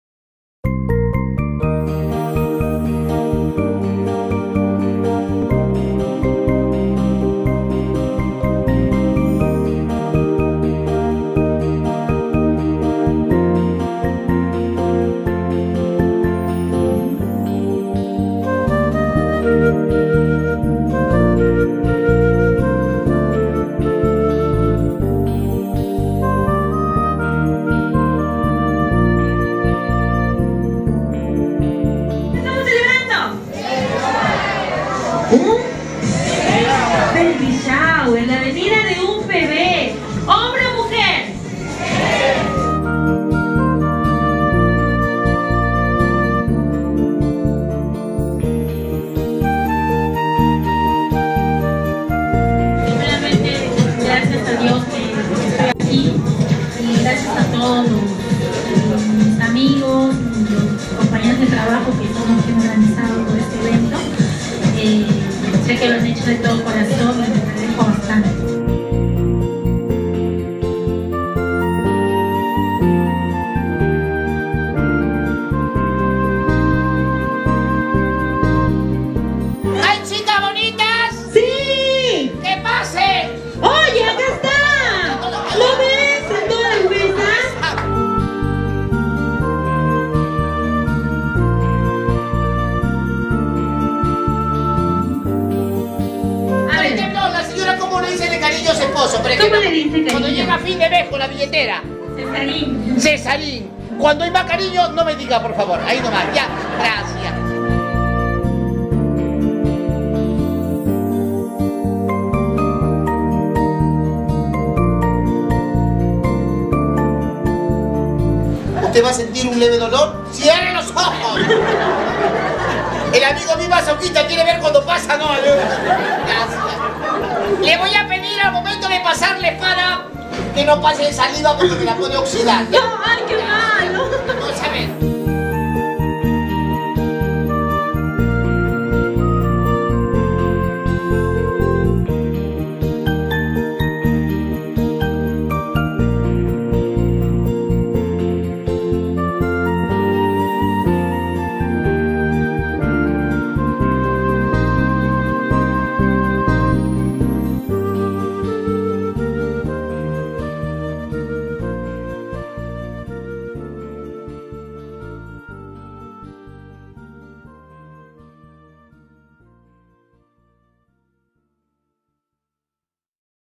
Bodas Finas y Mi kino soñado
Baby shower Abigail organización, decoración y show de payaso y animadora.